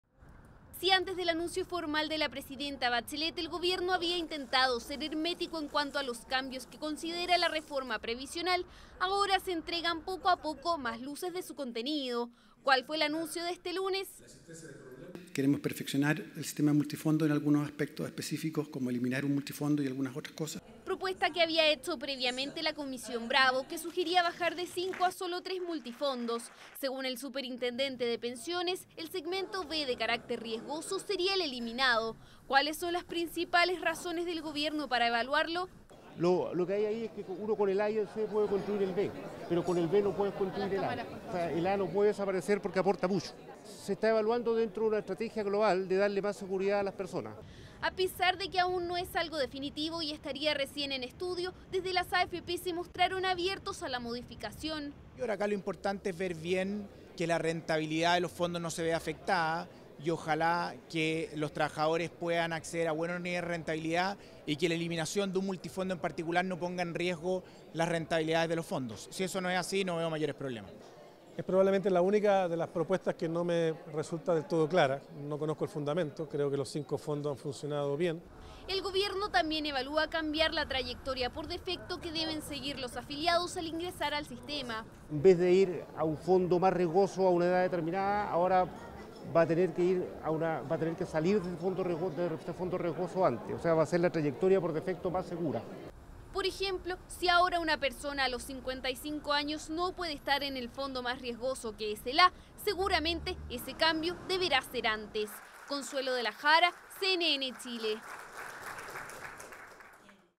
Pensiones: Evalúan eliminar el multifondo B
También proponen salir antes de fondos más riesgosos.